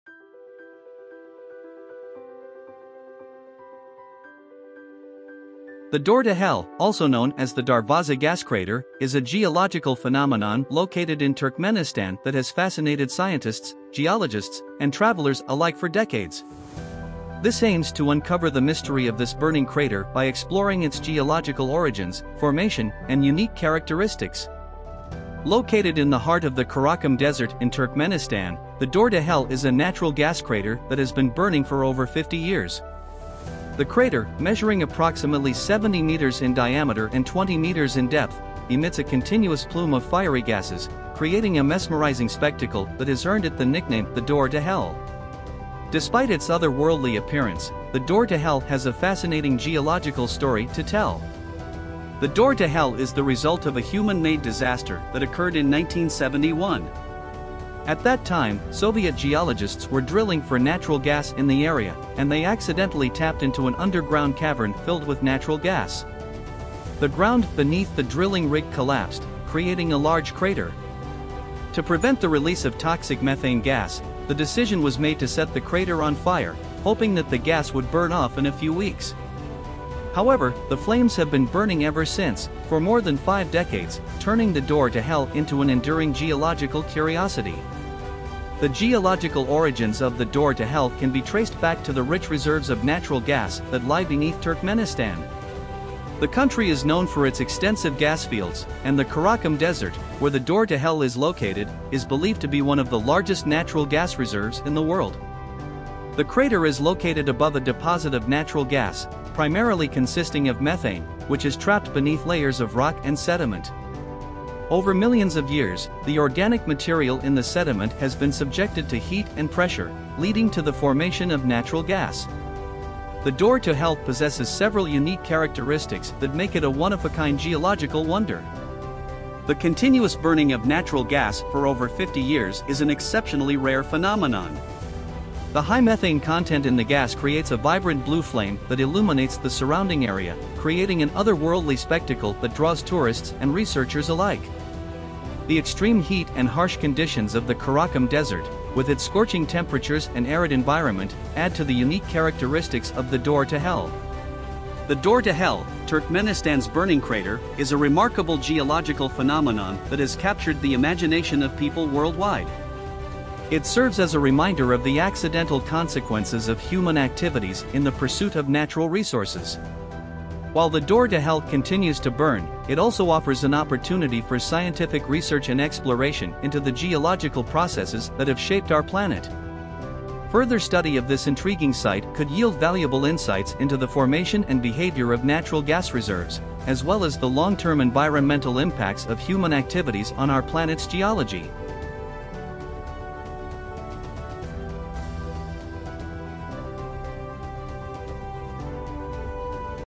The Door to Hell: Unveiling the Mystery of Darvaza Gas Crater
Welcome to a world of eternal flames and enigmatic wonders! In the heart of Turkmenistan's Karakum Desert lies a mesmerizing site known as the Door to Hell. With flames that have burned for over 50 years, this burning crater has intrigued explorers and researchers alike. Join us on an unforgettable journey as we uncover the geological mystery of the Door to Hell in this  video.